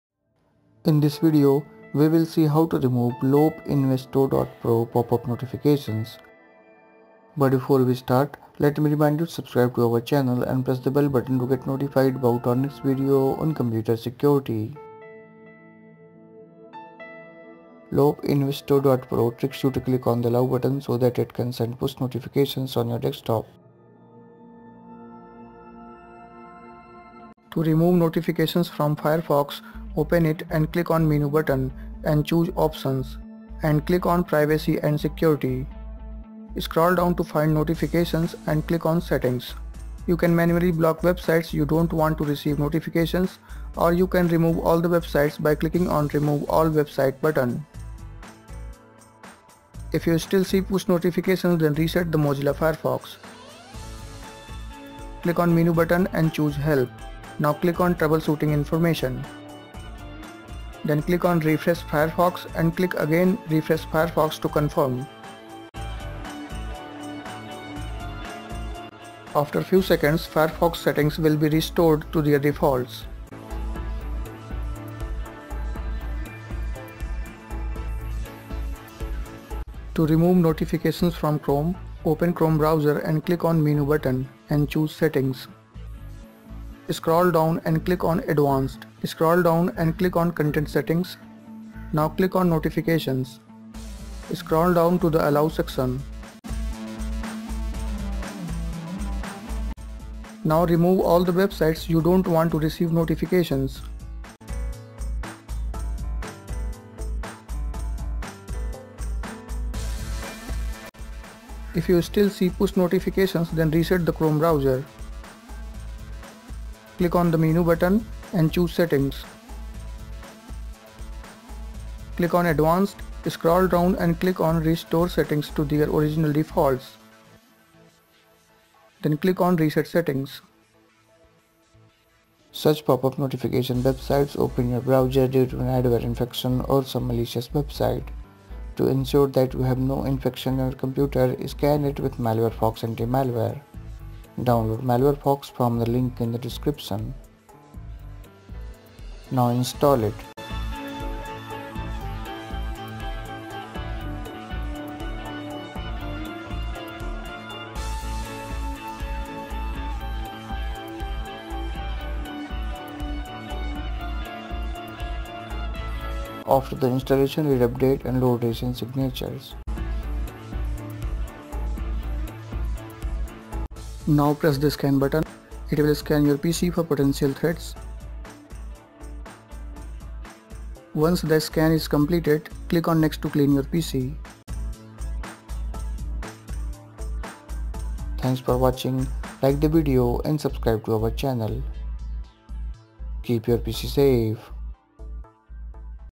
Remove Lopeinvesto.pro Pop-up Notifications (Firefox & Chrome)
Lopeinvesto.pro is a scam website that uses social engineering methods to trick users to click on the notification allow button. It asks to click on the notification allow button to start the streaming service. Once you click on the allow button, it starts sending push notifications on your computer. It affects browsers like Google Chrome, Mozilla Firefox, Internet Explorer, Microsoft Edge. In this video, how to remove Lopeinvesto.pro Pop-up notification, and you will know what is Lopeinvesto.pro. This video has Lopeinvesto.pro Push Notification removal guide.